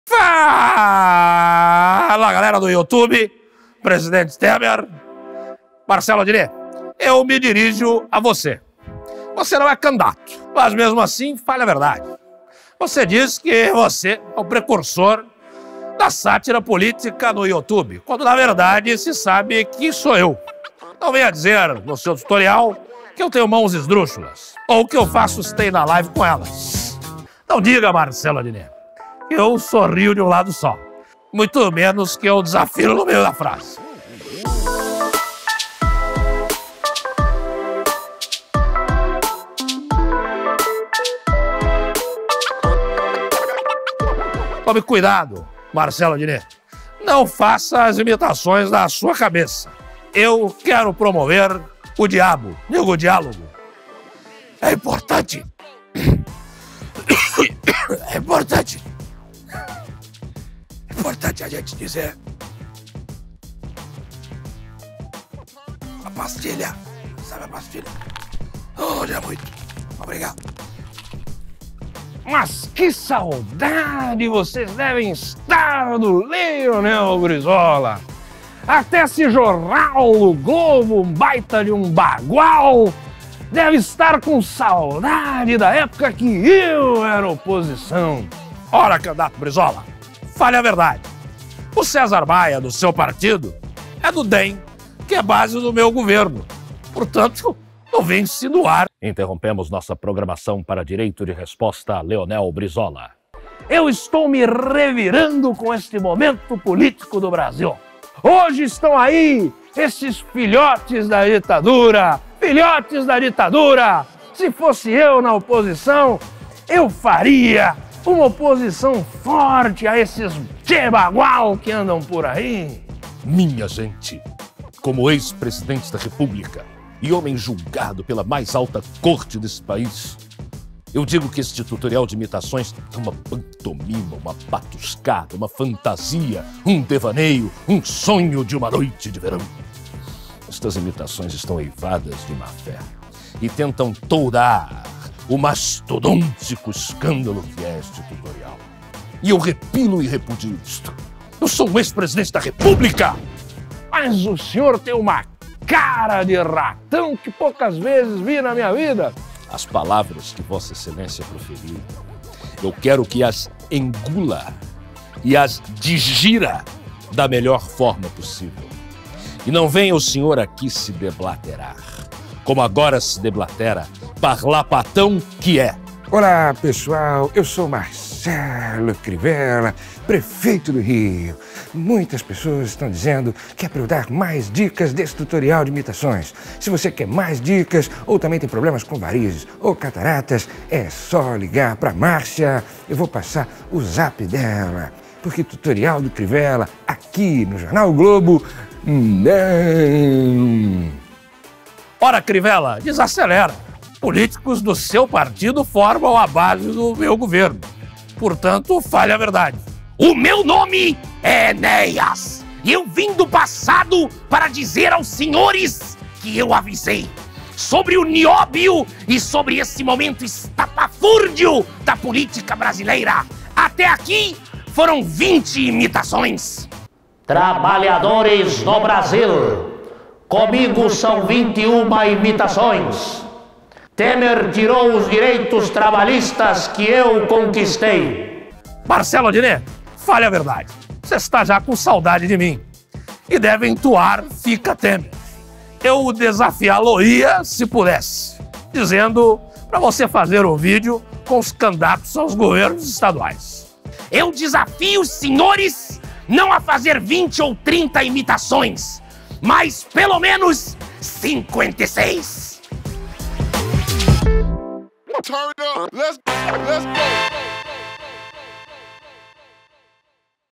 Marcelo Adnet imita Temer, Collor, Brizola, Crivella, Enéas e Vargas
Marcelo Adnet vai além das eleições 2018 e imita nomes já conhecidos da política brasileira, começando pelo presidente Michel Temer. O humorista também imita o ex-presidente Fernando Collor, o ex-governador do Rio de Janeiro Leonel Brizola, o prefeito do Rio, Marcelo Crivella (PRB), o ex-deputado federal Enéas Carneiro e o ex-presidente Getúlio Vargas.